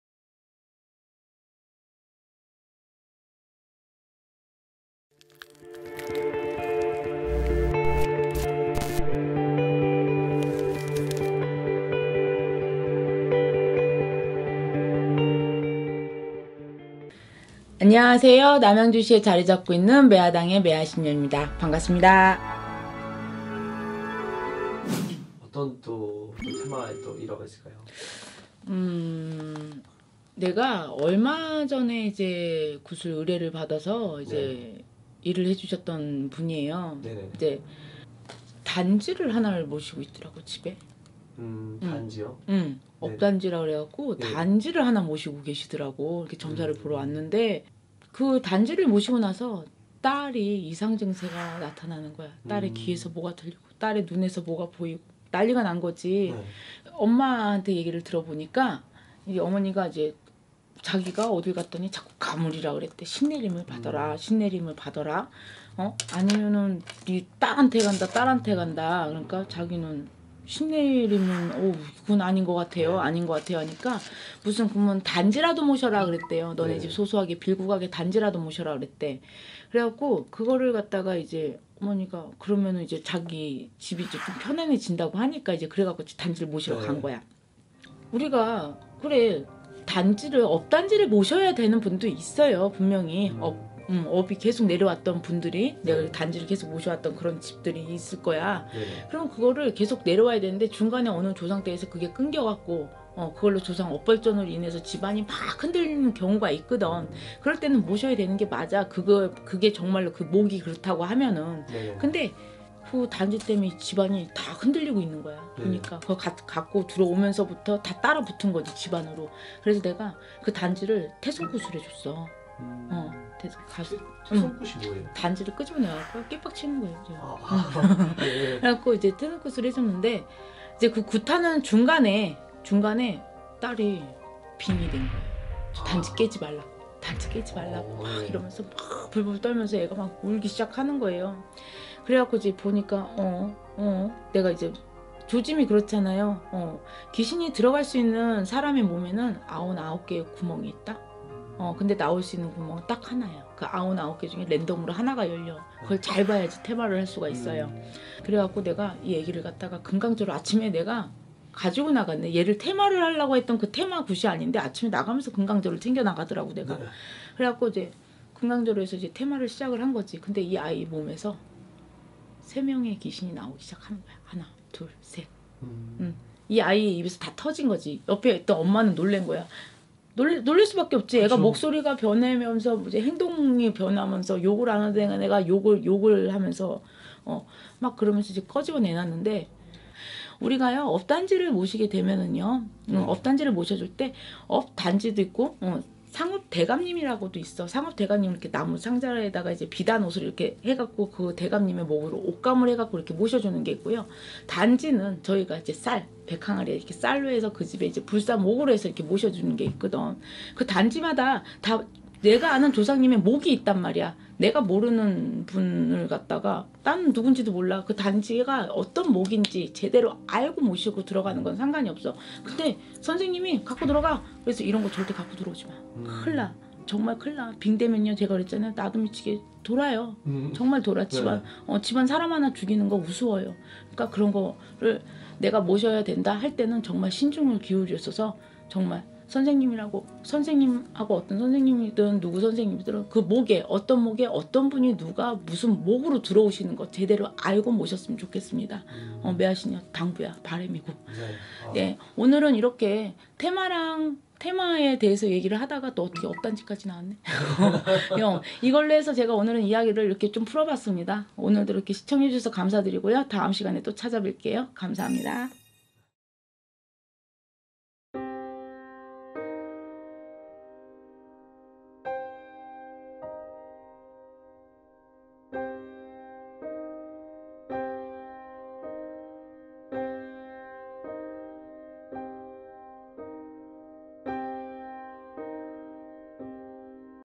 실제 퇴마사가 말하는 놀라운 퇴마사례와 귀신이 꼬이는 이유 [남양주 매화당]
상담가능시간 : 09:00 ~ 21:00